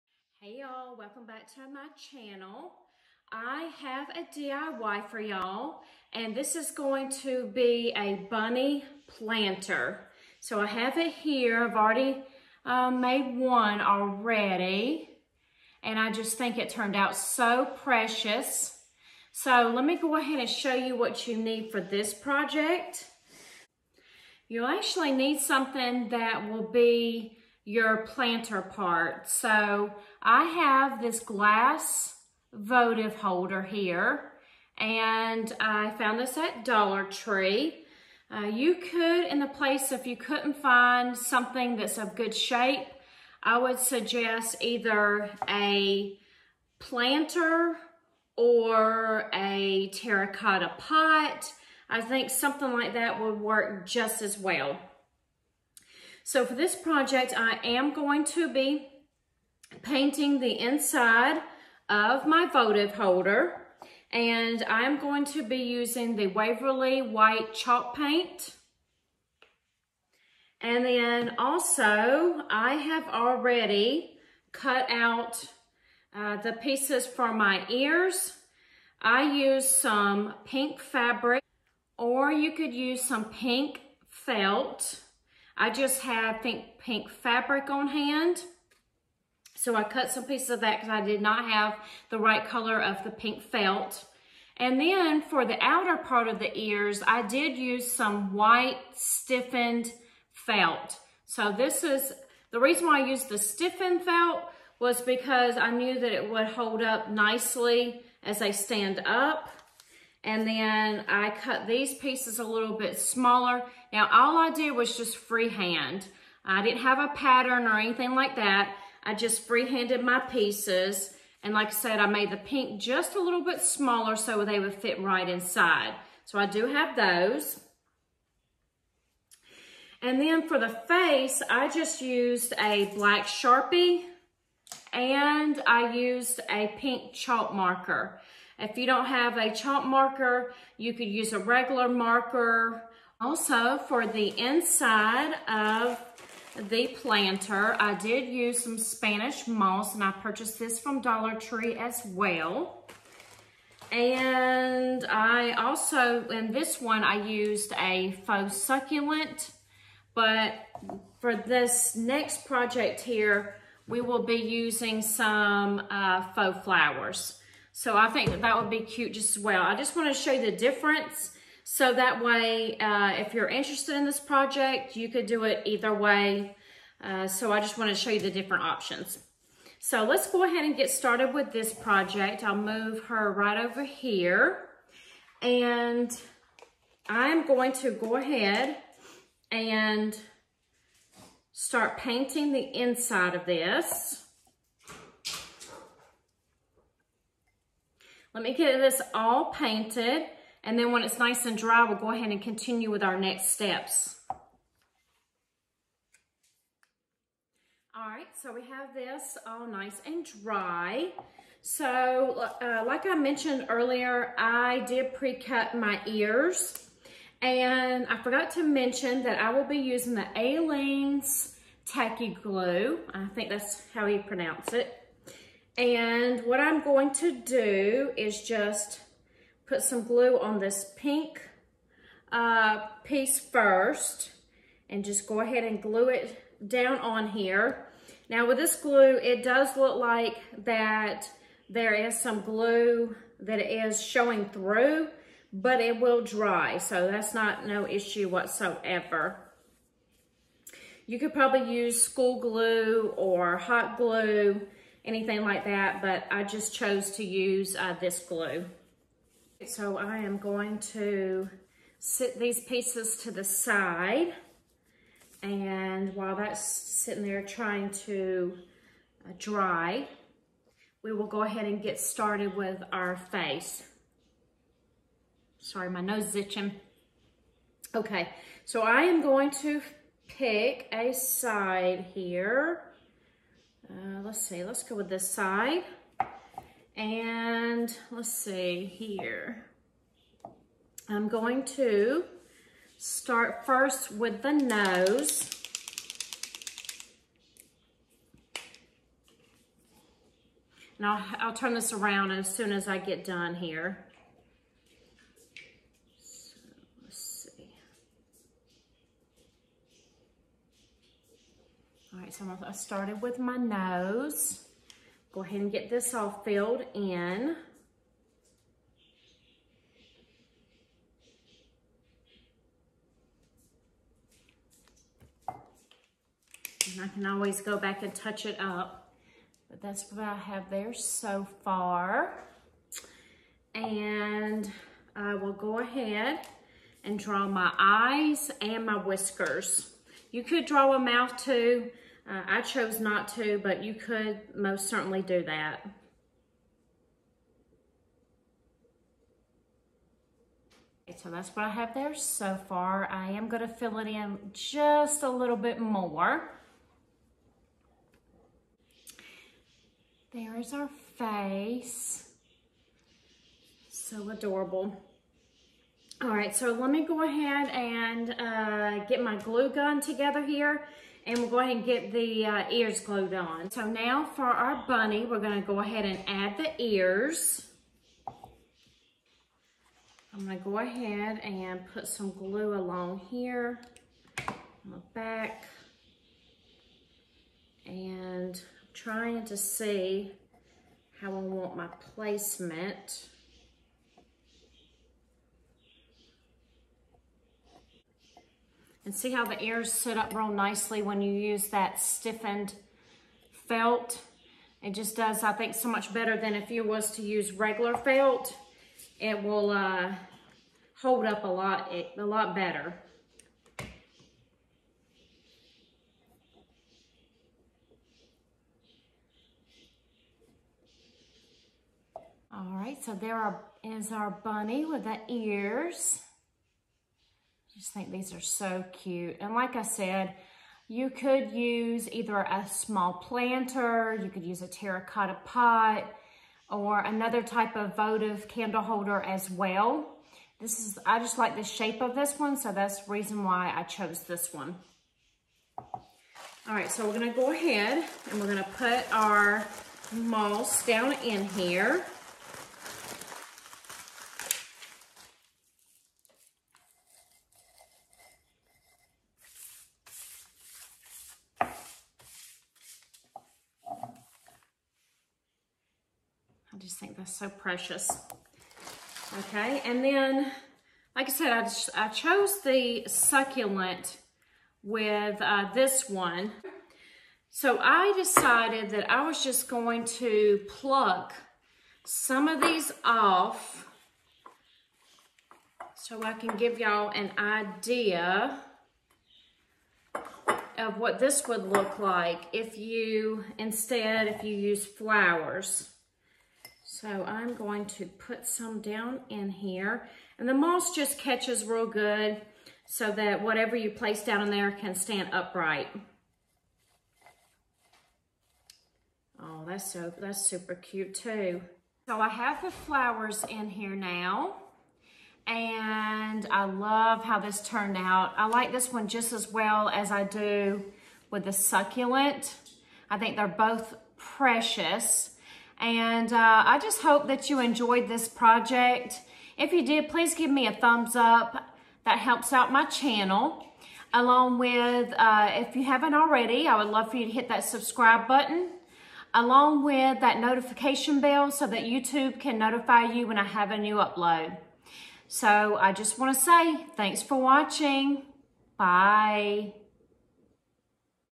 DIY Dollar Tree Bunny Planter
I have made a bunny planter with mostly all Dollar tree supplies.